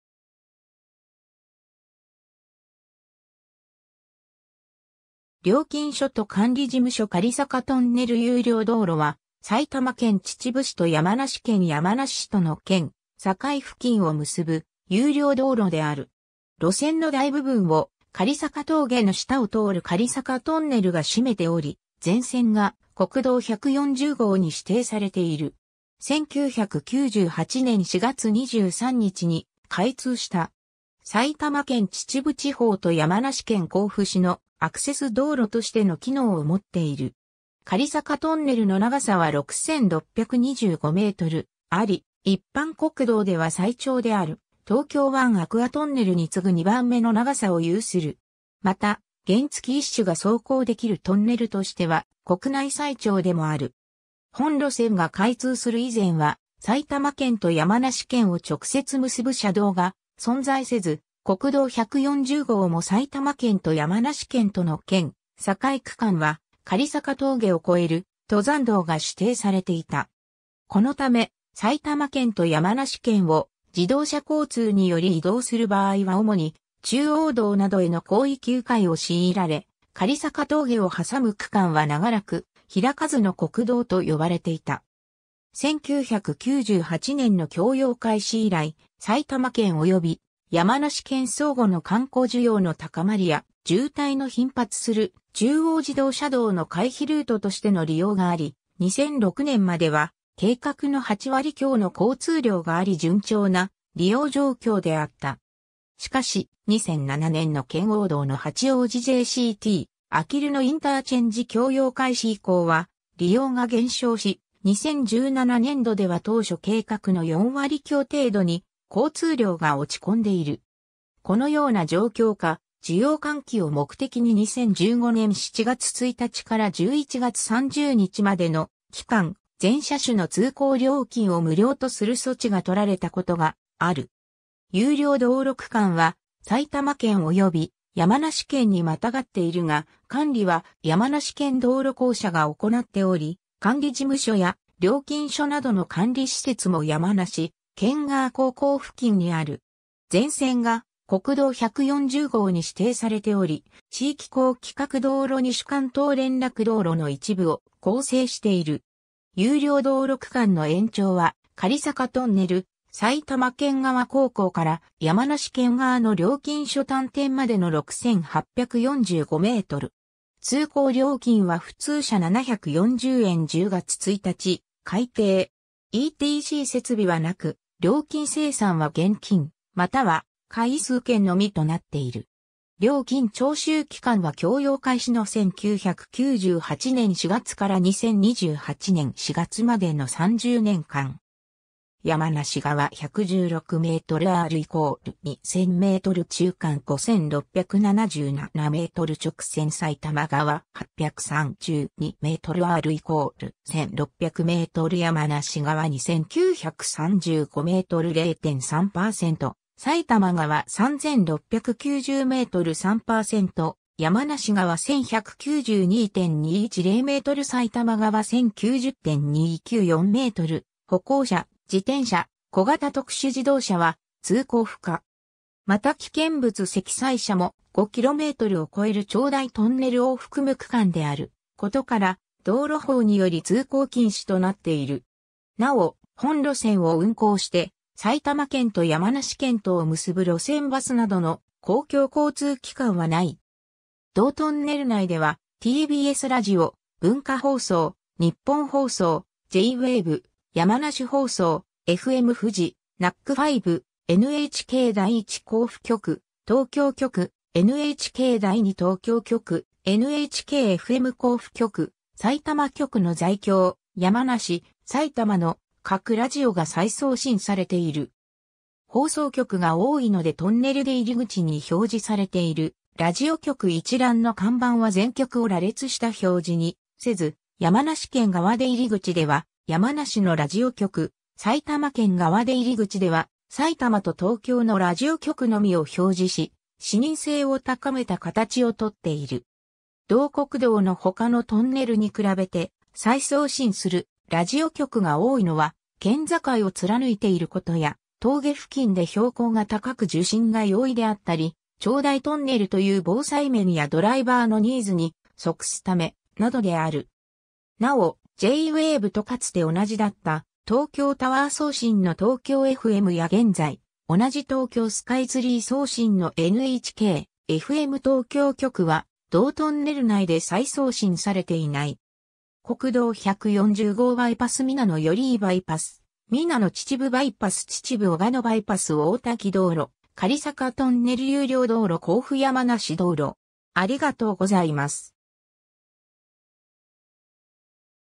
雁坂トンネル有料道路
料金所と管理事務所 雁坂トンネル有料道路（かりさかトンネルゆうりょうどうろ）は、埼玉県秩父市と山梨県山梨市との県境付近を結ぶ有料道路である。
路線の大部分を雁坂峠（標高：2,082m）の下を通る雁坂トンネルが占めており、全線が国道140号に指定されている。
1998年（平成10年）4月23日に開通した。
 埼玉県秩父地方と山梨県甲府市のアクセス道路としての機能を持っている。
雁坂トンネルの長さは6,625mあり、一般国道では最長である東京湾アクアトンネル（国道409号）に次ぐ2番目の長さを有する。
また、原付一種（50cc未満）が走行出来るトンネルとしては国内最長でもある。
 本路線が開通する以前は埼玉県と山梨県を直接結ぶ車道が存在せず、国道140号も埼玉県と山梨県との県境区間は雁坂峠を越える登山道（旧秩父往還）が指定されていた。
このため、埼玉県と山梨県を自動車交通により移動する場合は主に中央道などへの広域迂回を強いられ、雁坂峠を挟む区間は長らく「開かずの国道」と呼ばれていた。
 1998年（平成10年）の供用開始以来、埼玉県及び山梨県相互の観光需要の高まりや、渋滞の頻発する中央自動車道の回避ルートとしての利用があり、2006年（平成18年）までは、計画の8割強の交通量があり順調な利用状況であった。
しかし、2007年（平成19年）の圏央道の八王子JCT〜あきる野IC供用開始以降は利用が減少し、2017年度（平成29年度）では当初計画の4割強程度に交通量が落ち込んでいる。
このような状況下、需要喚起を目的に2015年（平成27年）7月1日から11月30日までの期間、全車種の通行料金を無料とする措置がとられたことがある。
 有料道路区間は埼玉県及び山梨県に跨っているが、管理は山梨県道路公社が行っており、管理事務所や料金所などの管理施設も山梨県側坑口付近にある。
全線が国道140号に指定されており、地域高規格道路西関東連絡道路の一部を構成している。
有料道路区間の延長は、雁坂トンネル埼玉県側坑口から山梨県側の料金所端点までの6,845m。
通行料金は普通車740円（2019年（令和元年）10月1日改定）。
ETC設備はなく、料金精算は現金または回数券のみとなっている。
料金徴収期間は供用開始の1998年（平成10年）4月から2028年（令和10年）4月までの30年間。
 山梨側　116m R=2000m 中間　5677m 直線 埼玉側　832m R=1600m 山梨側　2935m 0.3% 埼玉側　3690m 3% 山梨側　1192.210m 埼玉側　1090.294m 歩行者・自転車・小型特殊自動車は通行不可（原付50 ccは通行可）。
また危険物積載車も、5 kmを超える長大トンネルを含む区間であることから、道路法により通行禁止となっている。
なお、本路線を運行して埼玉県と山梨県とを結ぶ路線バスなどの公共交通機関はない。
 同トンネル内ではTBSラジオ、文化放送、ニッポン放送、J-WAVE、山梨放送、FM-FUJI、NACK5、NHK第一甲府局・東京局、NHK第二東京局、NHK-FM甲府局・さいたま局の在京・山梨・埼玉の各ラジオが再送信されている。
放送局が多いのでトンネル出入口に表示されているラジオ局一覧の看板は全局を羅列した表示にせず、山梨県側出入口では山梨のラジオ局、埼玉県側出入口では埼玉と東京のラジオ局のみを表示し、視認性を高めた形をとっている。
同国道の他のトンネルに比べて再送信するラジオ局が多いのは、県境を貫いていることや峠付近で標高が高く受信が容易であったり、長大トンネルという防災面やドライバーのニーズに即すため、などである。
なお、J-WAVEとかつて同じだった東京タワー送信のTOKYO FMや現在同じ東京スカイツリー送信のNHK-FM東京局は同トンネル内で再送信されていない。
 （熊谷方面）国道140号バイパス - 皆野寄居バイパス ‐ 皆野秩父バイパス - 秩父小鹿野バイパス - 大滝道路 - 雁坂トンネル有料道路 - 甲府山梨道路（甲府方面）